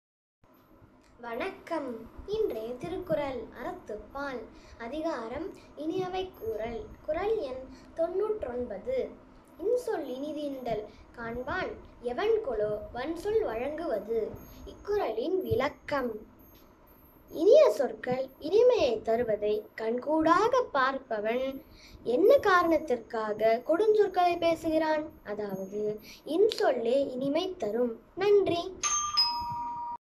குறள் எண் - 99 திருக்குறள். அறத்துப்பால். அதிகாரம் - 10 இனியவை கூறல்.
இன்சொல் இனிதீன்றல் காண்பான் எவன்கொலோ
வன்சொல் வழங்கு வது